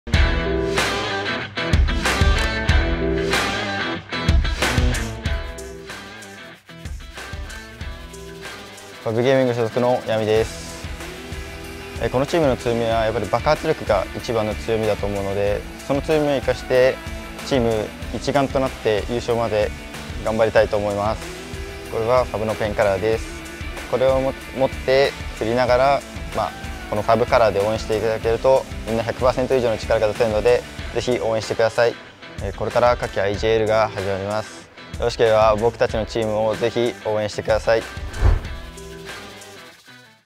【2024夏季IJL】新シーズンスピーチ FAV gaming
新しいシーズンに向けての意気込みを、
チームの大切なアイテムと共に皆さんへ伝えます！

選手はどんなアイテムを持ってきたのか？！